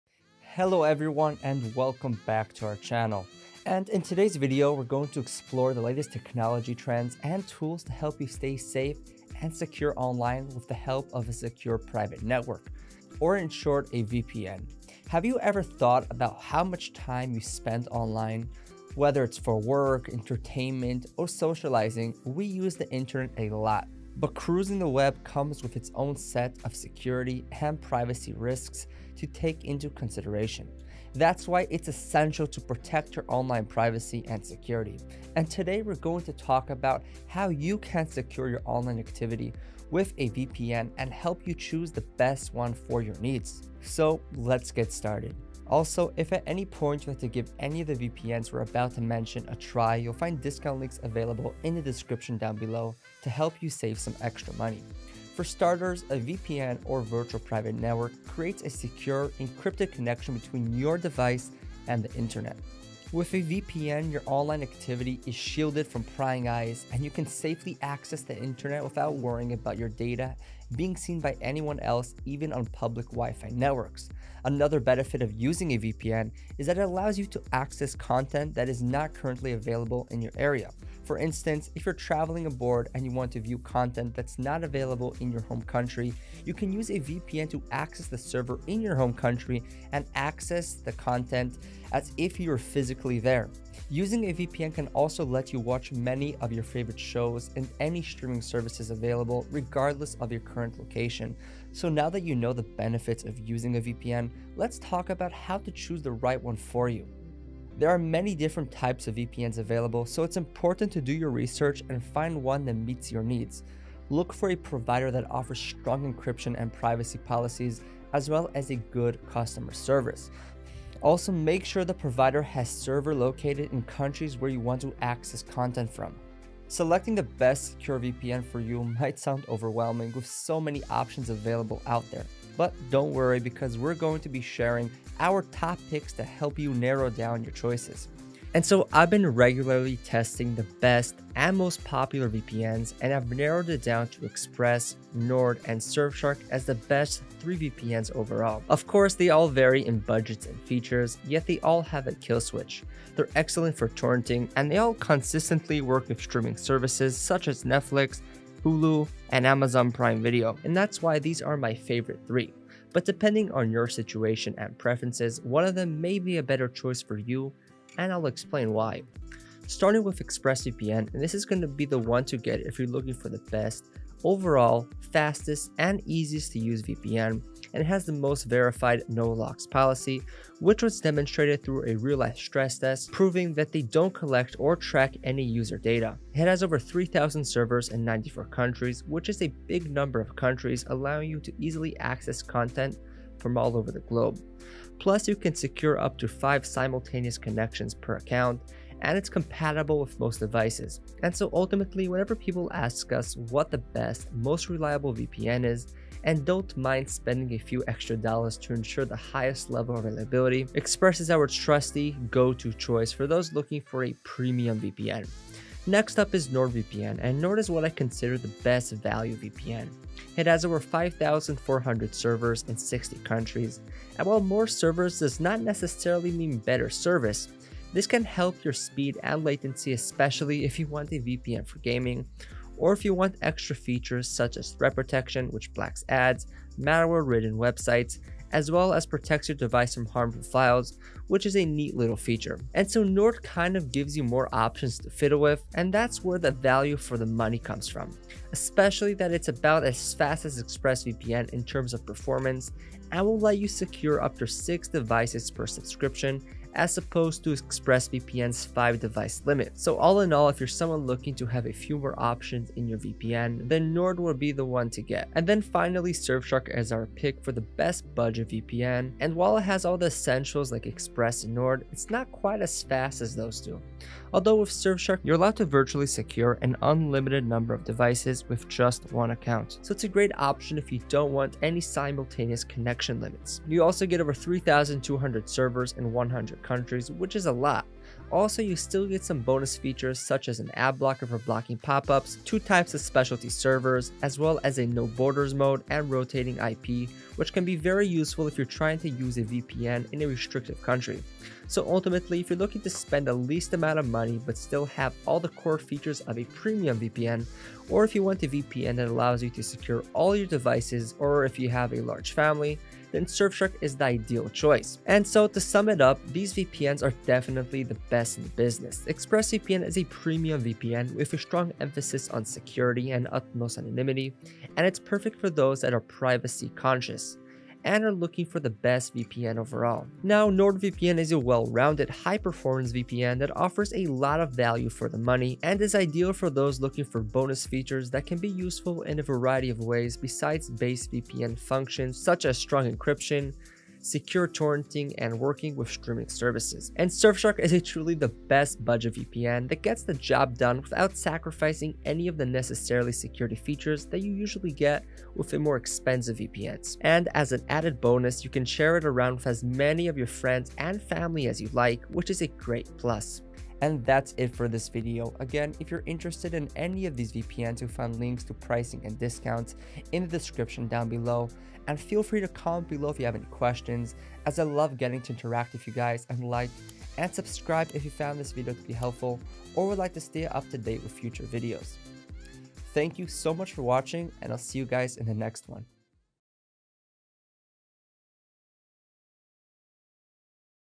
Protect Your Online Security with a Secure VPN
Protect your online security with a secure VPN. Stay safe while browsing the internet with a secure VPN. Get the ultimate security and privacy with fast and reliable VPN connection.

And in today's video, we're going to explore the latest technology trends and tools to help you stay safe and secure online with the help of a secure private network, or in short, a VPN. Have you ever thought about how much time you spend online, whether it's for work, entertainment, or socializing, we use the internet a lot. But cruising the web comes with its own set of security and privacy risks to take into consideration. That's why it's essential to protect your online privacy and security. And today, we're going to talk about how you can secure your online activity with a VPN and help you choose the best one for your needs. So let's get started. Also, if at any point you have to give any of the VPN for a bounce and mention a try, you'll find discount links available in the description down below to help you save some extra money. For starters, a VPN or virtual private network creates a secure encrypted connection between your device and the internet. With the VPN your online activity is shielded from prying eyes and you can safely access the internet without worrying about your data being seen by anyone else, even on public Wi Fi networks. Also make sure the provider has server located in countries where you want to access content from selecting the best your VPN for you might sound overwhelming with so many options available out there. Now Nord VPN is a well rounded, high performance VPN that offers a lot of value for the money and is ideal for those looking for bonus features that can be useful in a variety of ways. Besides basic VPN functions such as strong encryption, secure torrenting, and working with streaming services and surf shark is a truly the best budget VPN that gets the job done without sacrificing any of the necessarily security features that you usually get with a more expensive VPS. And as an added bonus, you can share it around for as many of your friends and family as you'd like, which is a great plus. And that's it for this video again, if you're interested in any of these VPN to find links to pricing and discounts in the description down below. And feel free to comment below if you have any questions as I love getting to interact with you guys and like and subscribe if you found this video to be helpful or would like to stay up to date with future videos.

Hope you enjoyed my Protect Your Online Security with a Secure VPN Video.